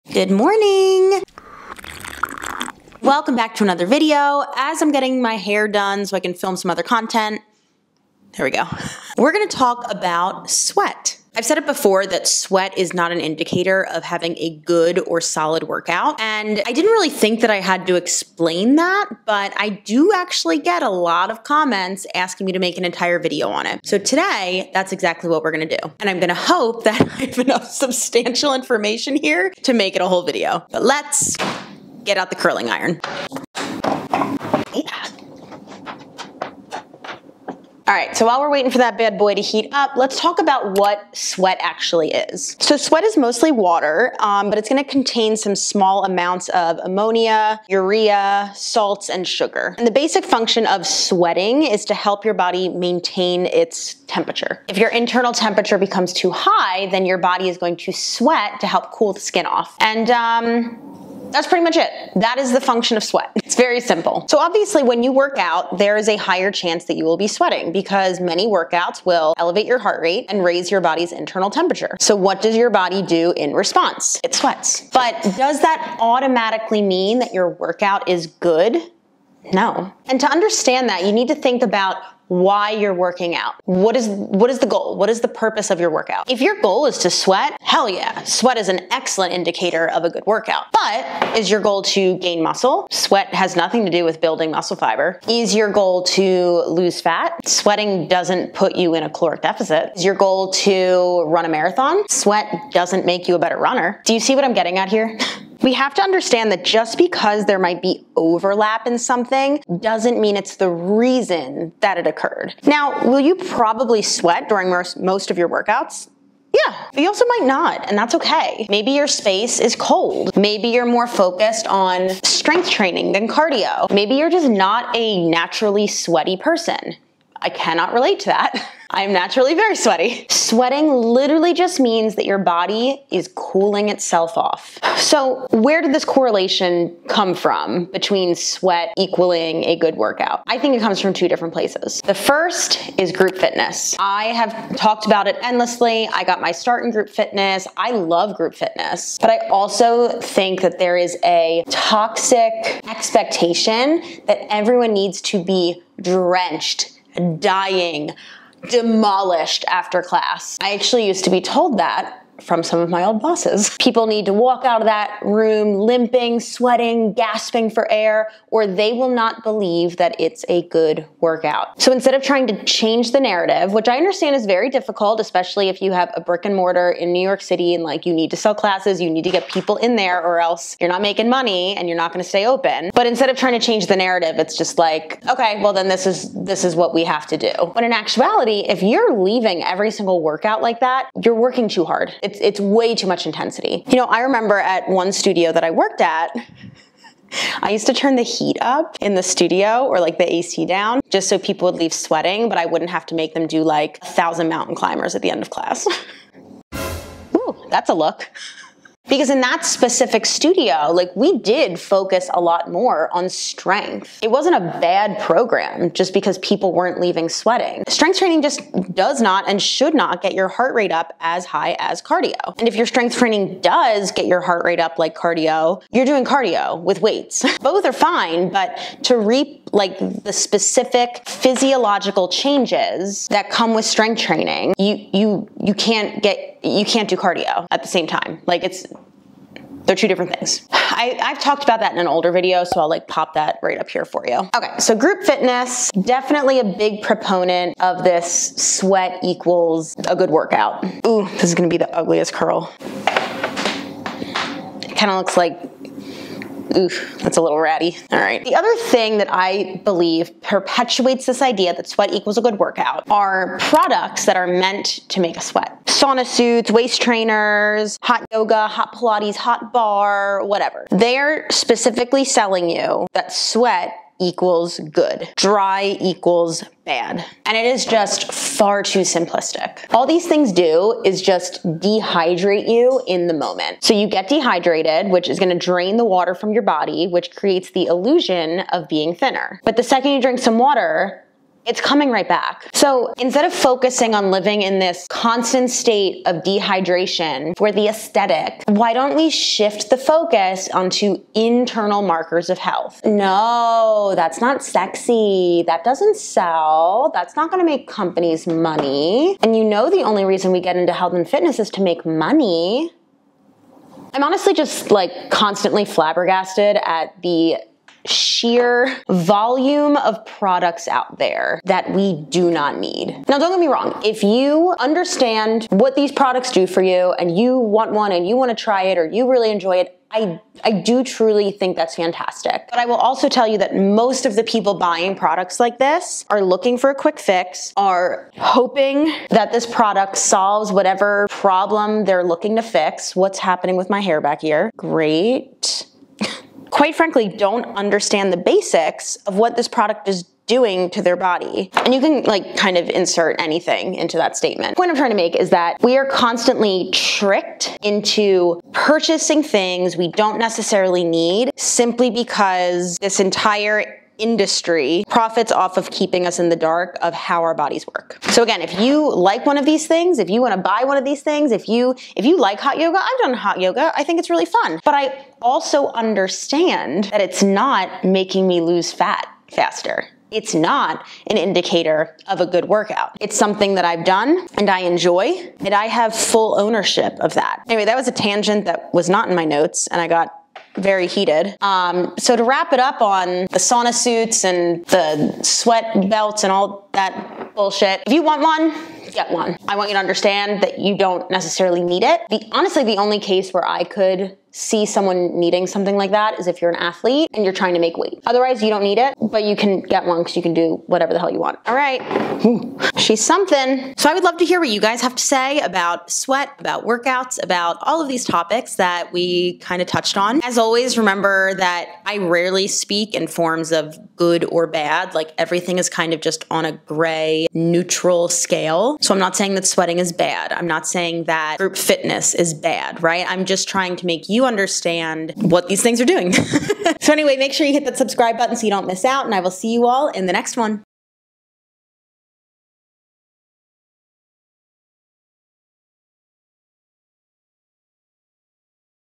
Sweating is overrated.
Hi everybody! Today we’re talking all things sweat and why I think the industry has forced it down our throats.